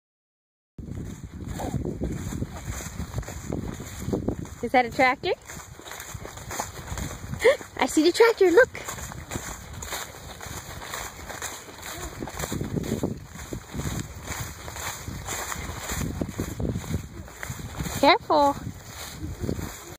Child Running Through a Wheat Field
In the wheat field watching an old John Deere plow...